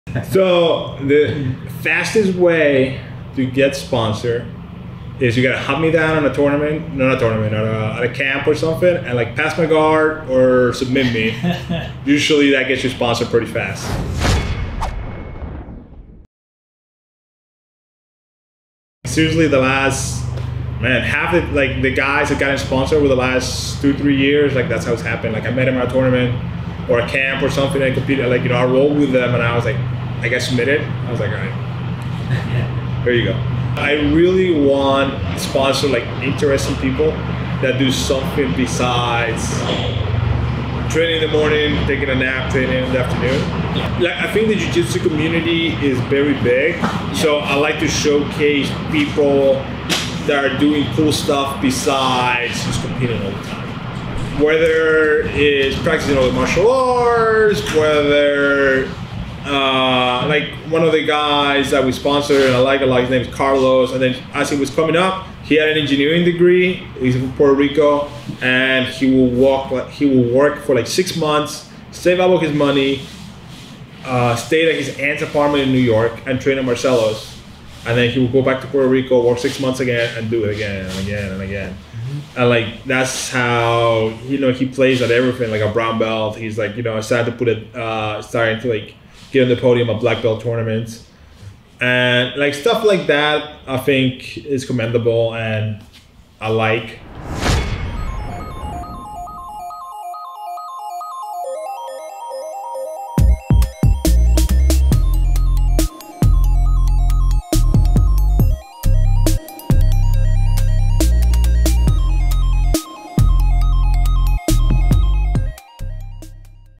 How to get Sponsored by Inverted Gear
In this video Nelson talks about how he decides who to sponsor and the easiest way you can get a sponsorship from Inverted Gear.